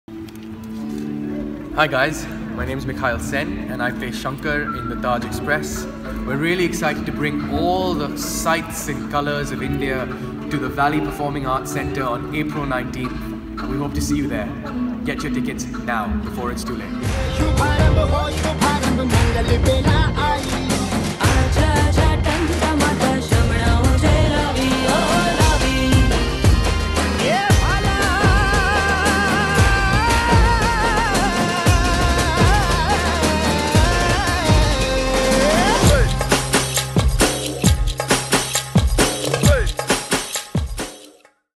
Mikhail Sen Invites You to Taj Express: The Bollywood Musical Revue
Actor Mikhail Sen invites you to go on a live cinematic journey through modern Indian culture with Taj Express: The Bollywood Musical Revue at VPAC on Apr 19! Limited tickets left

Wed April 19 at 8PM
Taj Express: The Bollywood Musical Revue

Taj Express explodes with the sounds of India and Bollywood, capturing the vibrant, expressive spirit of the world of Bollywood movies that have been entertaining billions of people in India for generations. Through a fusion of film, dance, and music, this dazzling international sensation will take audiences on a live cinematic journey through modern Indian culture and society during their 2017 US tour. The production is a high energy celebration of new India’s pop music, Bollywood culture, and deep traditions featuring colorful costumes, joyful dance, and thrilling live music.